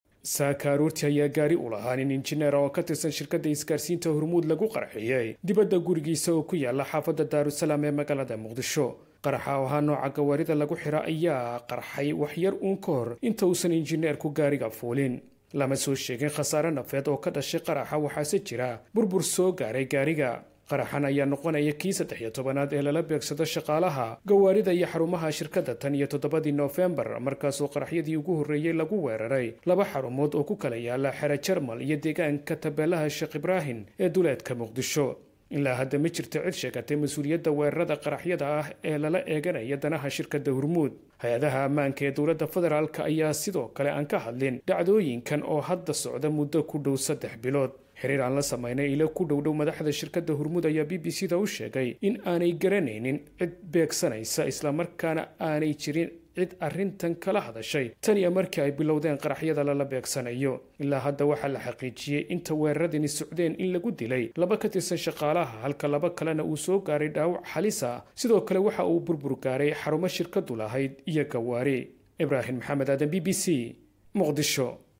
faahfaahinta qaraxyadii lala beegsaday xarumaha iyo shaqaalaha shirkadda isgaarsiinta Hormuud
Dhageyso faahfaahinta qaraxyadii lala beegsaday xarumaha iyo shaqaalaha shirkadda isgaarsiinta Hormuud